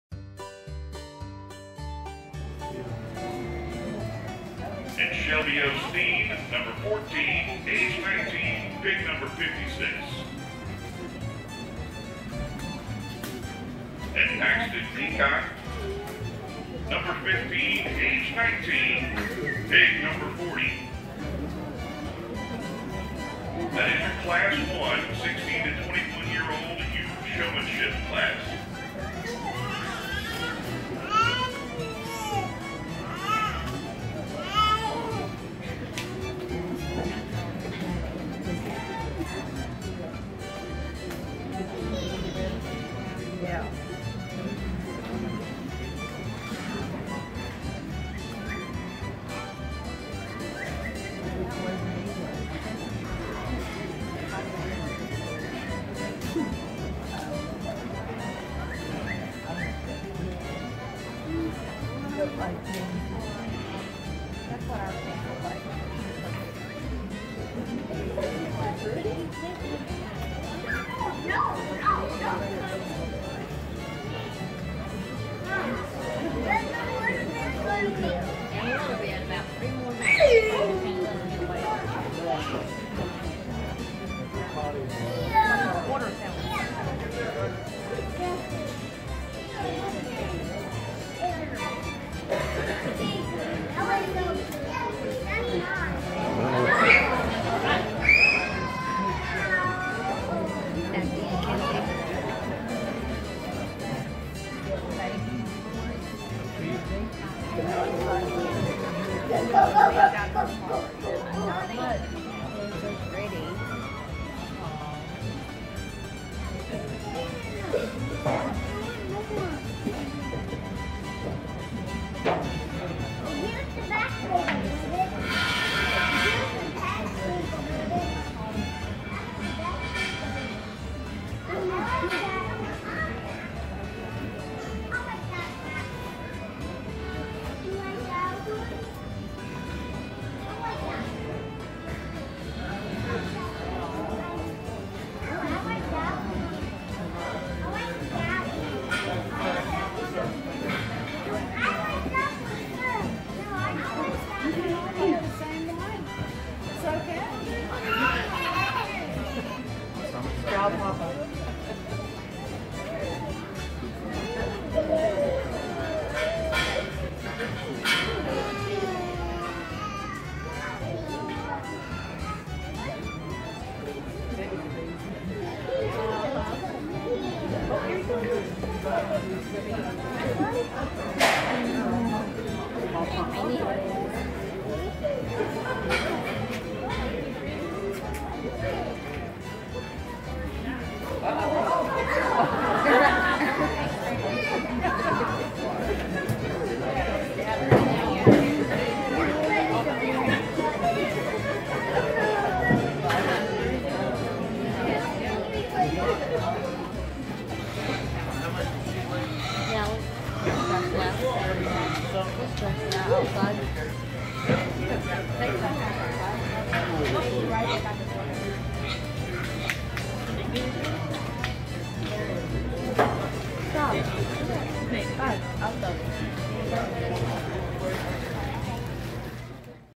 Wakulla County Swine Show - Showmanship - February 17, 2018
Clips from the Showmanship classes from today's swine show at the Wakulla County Livestock Pavilion.  pig, hog, swine